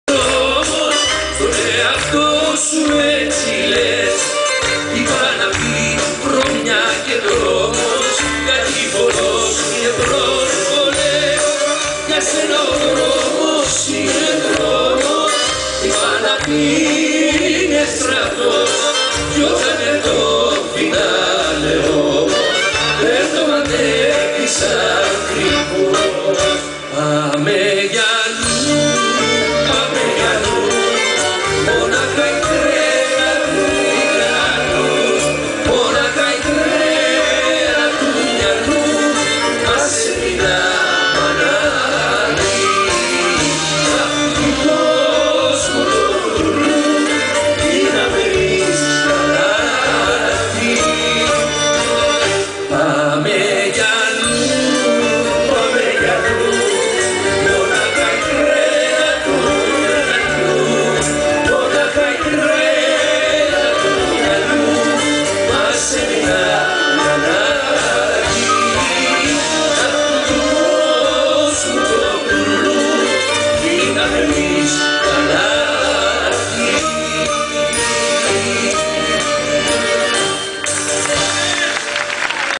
Giorgos Ntalaras, Despina Olympioy kai Mixalis Tsouganakis part 2
Giorgos Ntalaras, Despina Olympioy kai Mixalis Tsouganakis sthn Kypro, Germasogeias 2010